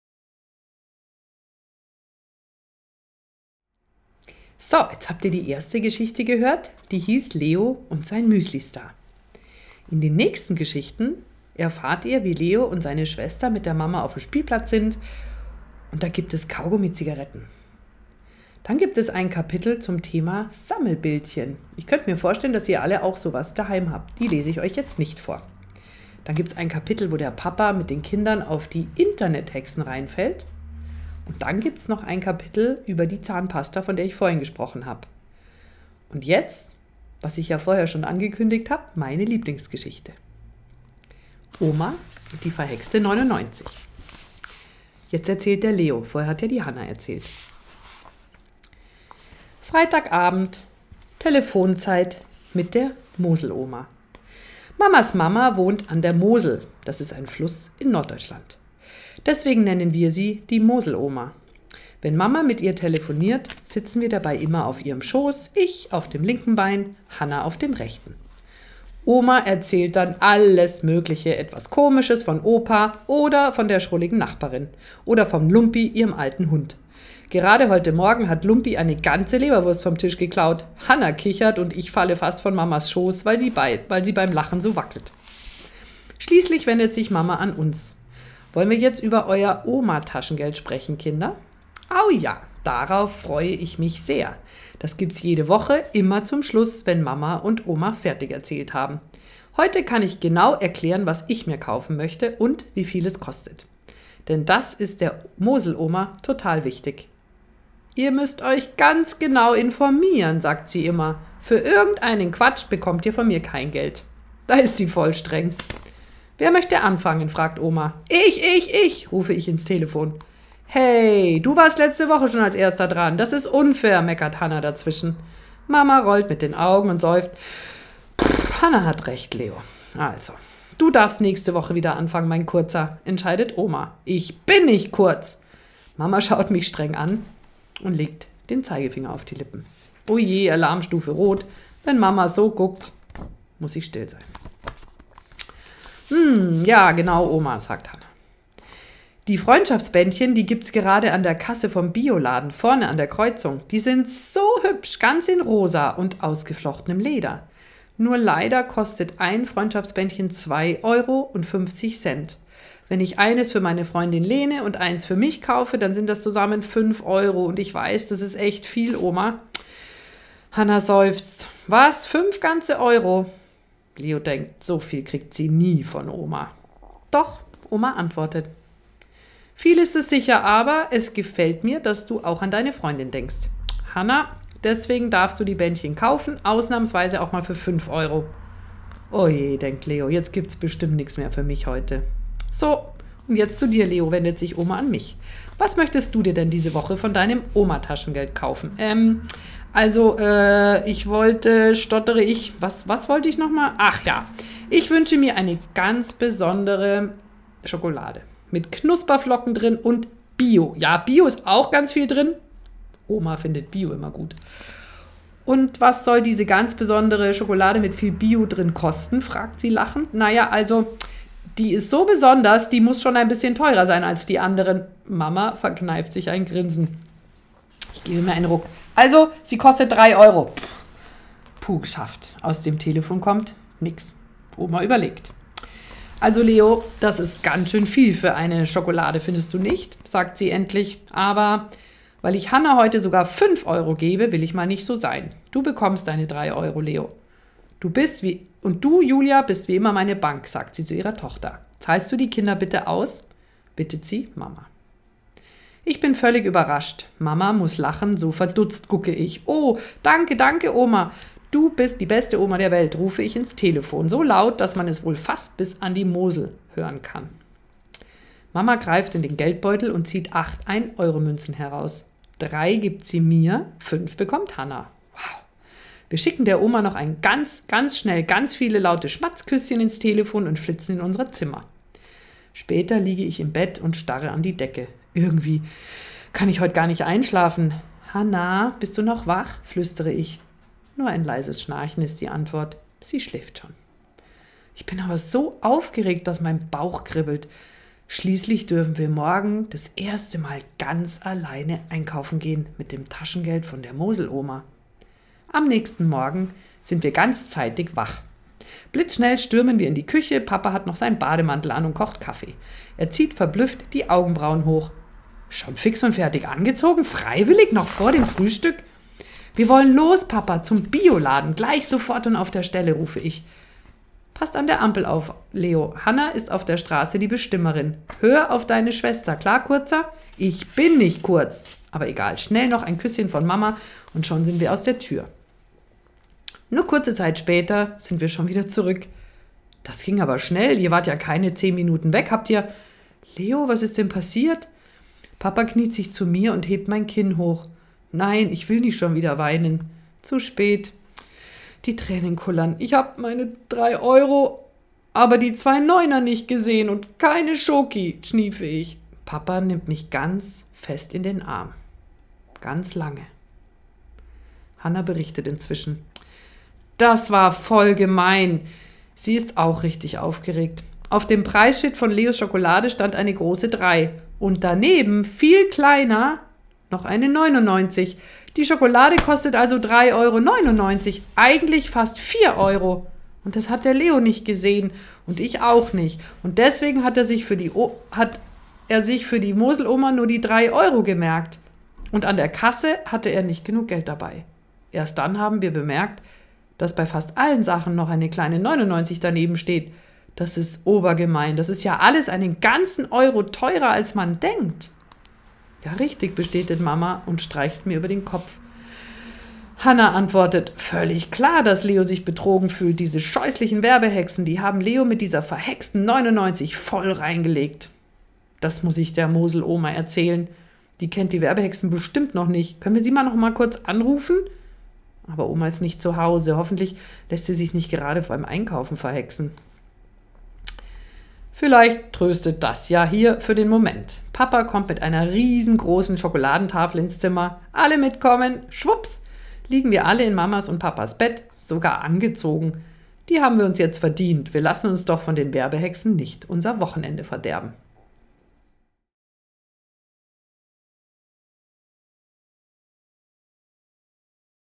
Karin Burger – Werbehexen – Lesung Teil 2: Oma und die verhexte 99
Im zweiten Teil liest Karin Burger aus dem Kapitel "Oma und die verhexte 99".

Die Lesung ist eine gemeinsame Produktion der Redaktion Freistunde und der VHS Straubing.